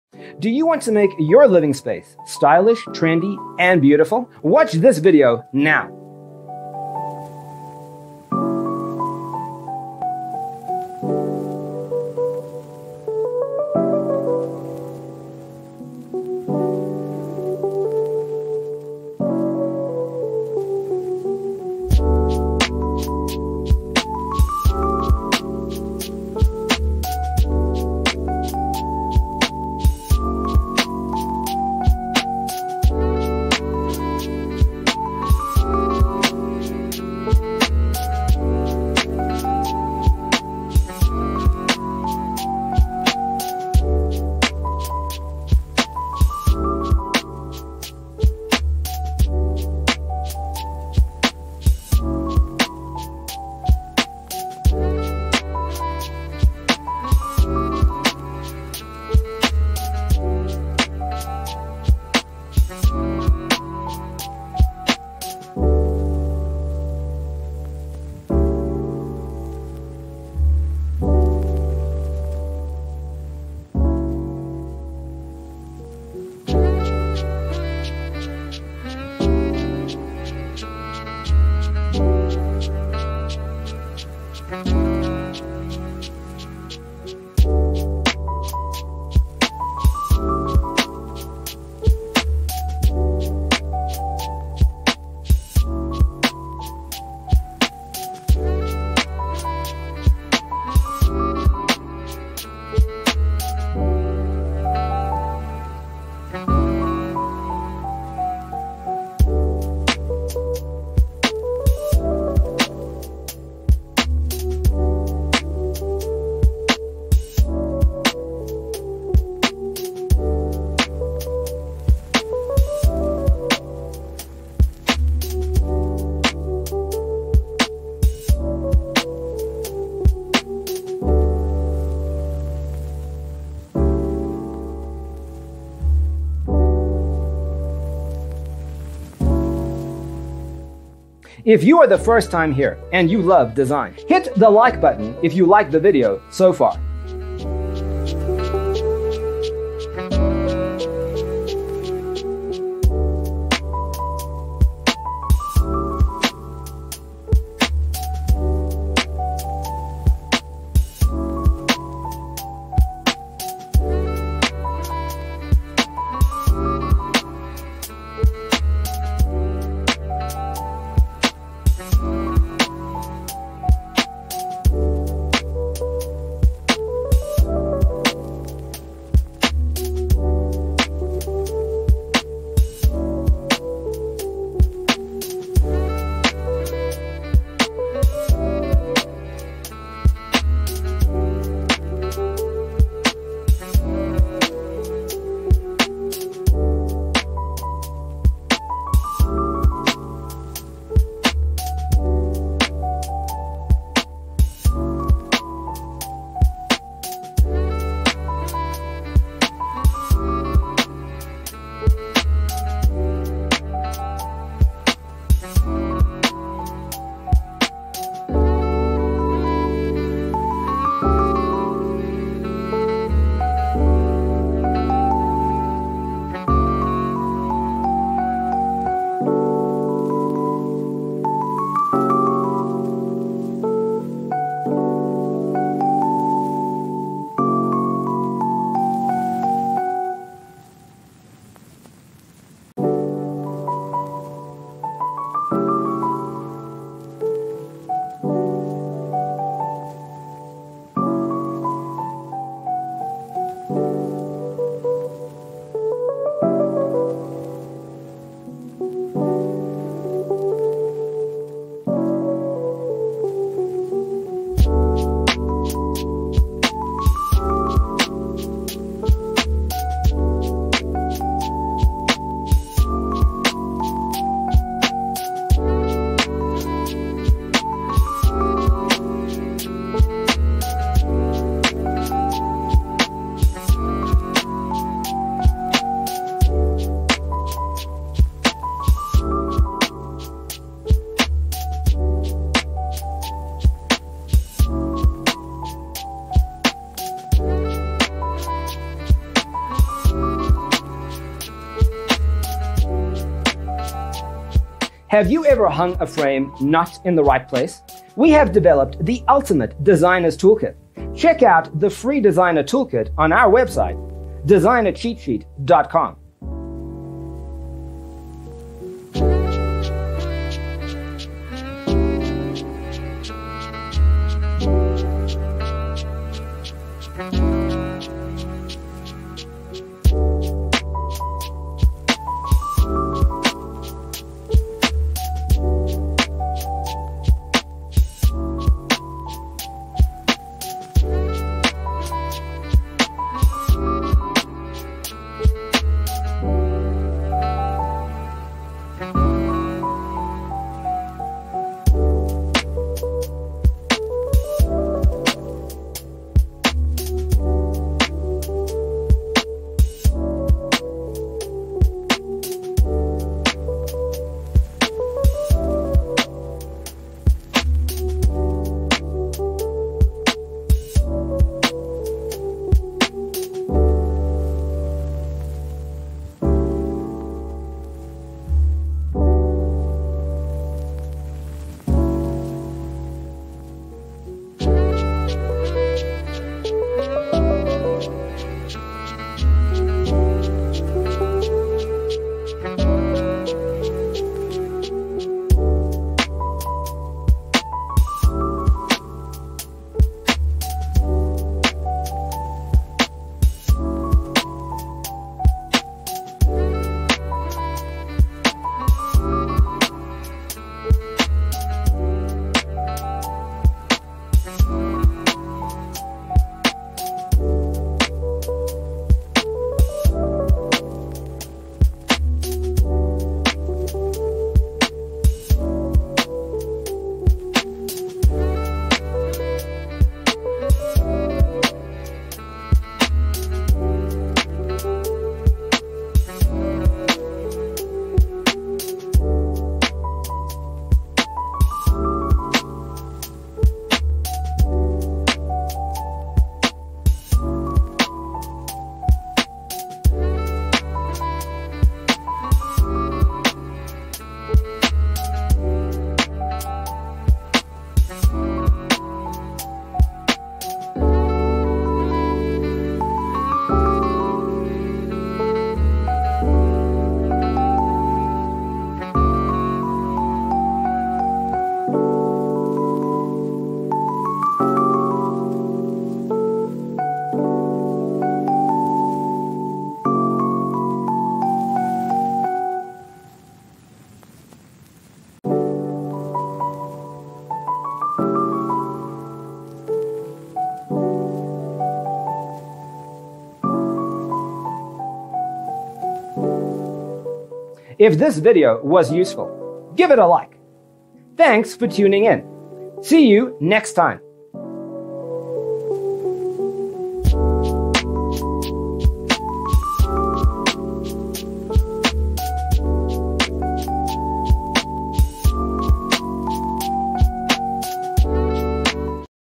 75 Gray Kitchen With Beige Backsplash Design Ideas You'll Love 🌈
Browse photos of gray kitchen designs. Discover inspiration for your gray kitchen remodel or upgrade with ideas for storage, organization, layout and decor.

0:00:00 - Intro
0:00:12 - Gray Kitchen With Beige Backsplash Design Ideas You'll Love: Inspirations 1 to 25
0:03:03 - Gray Kitchen With Beige Backsplash Design Ideas You'll Love: Inspirations 26 to 50
0:06:07 - Gray Kitchen With Beige Backsplash Design Ideas You'll Love: Inspirations 51 to 75

Hello there!🌈